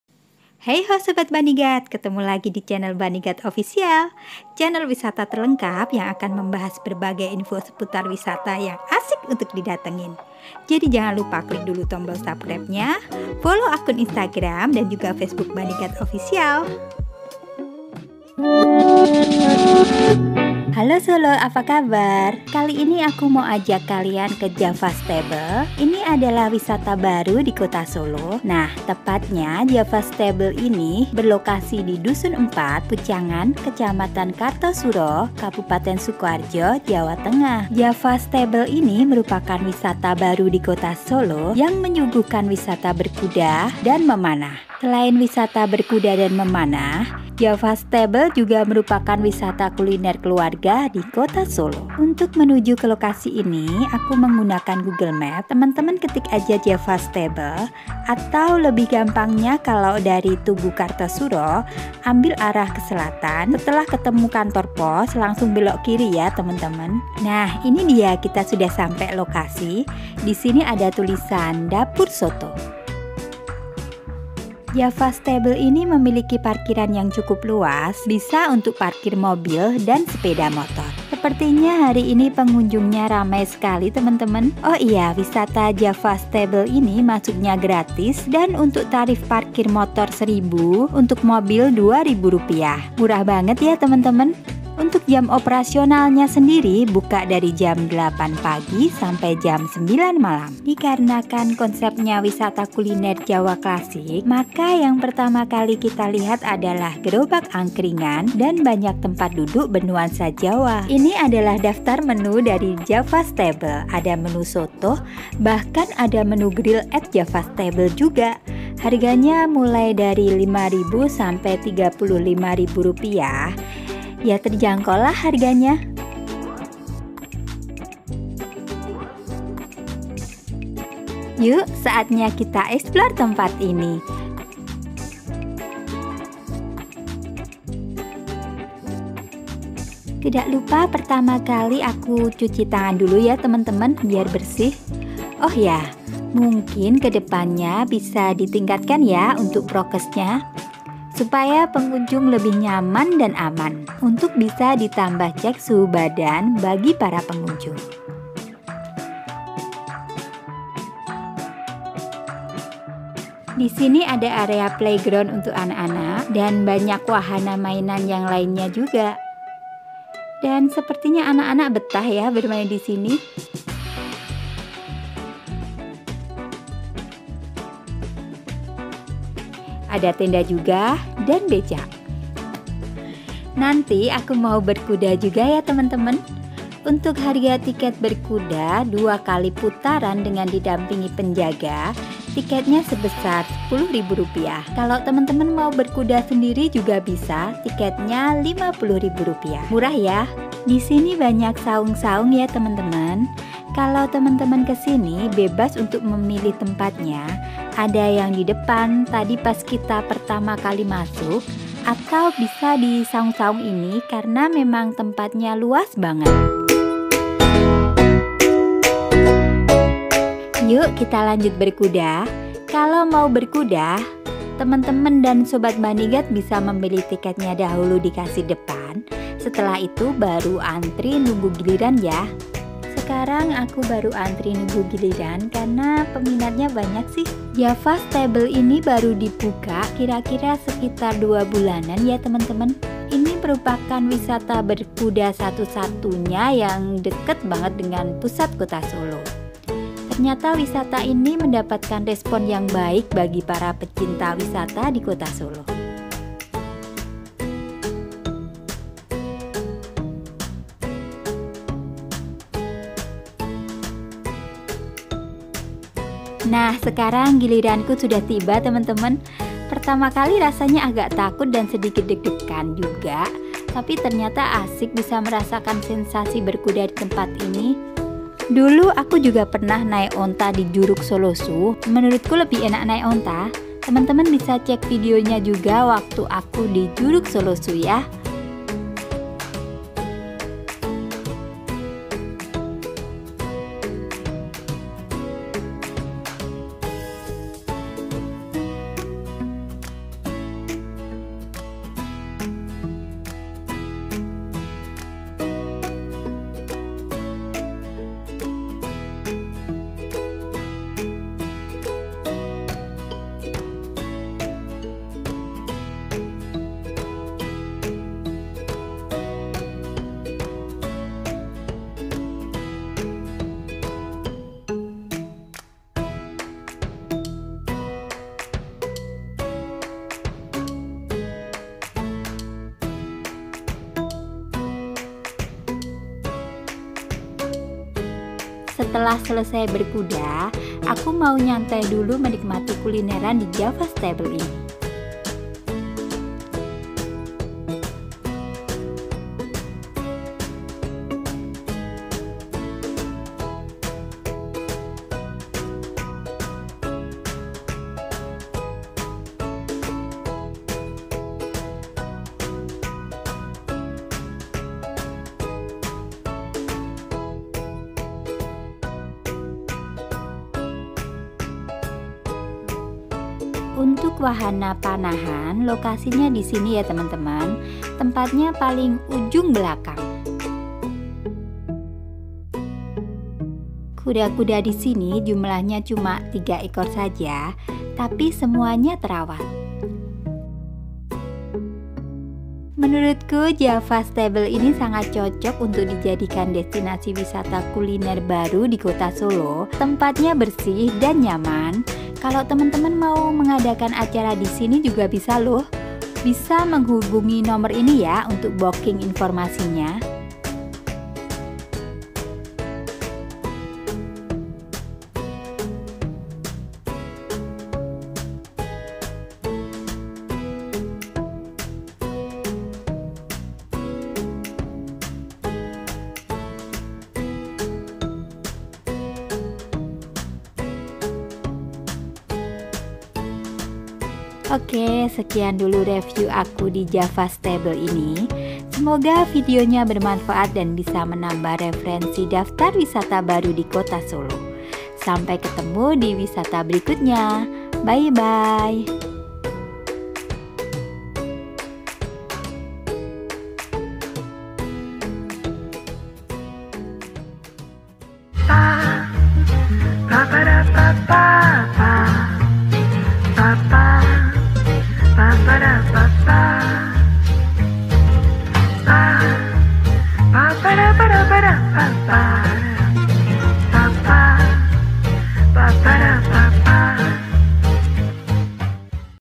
JAVA STABLE WISATA BERKUDA DI SOLO || Wisata Baru di Kartasura Solo 2022 ‼️
Wisata baru di solo kali ini adalah Java Stable wisata berkuda di solo, ini merupakan wisata yang baru ngehits yang menyuguhkan wisata berkuda dan memanah, selain itu JAVA stable juga merupakan wisata kuliner keluarga di solo.
Java stable wisata berkuda di solo ini baru dibuka sekitar dua bulanan,jadi masih tergolong wisata baru di solo 2022.Java stable berlokasi di Dusun IV, Pucangan, Kec. Kartasura, Kabupaten Sukoharjo, Jawa Tengah.
Harga Tiket Masuk Java Stable Solo gratis, dan untuk jam operasionalnya dibuka dari jam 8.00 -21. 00 wib.
Bagaimana keseruan Java stable wisata berkuda di solo ini? Yuk tetap simak terus videonya sampai akhir ya temen - temen dan sobat banigad. Happy watching😍😍😍😍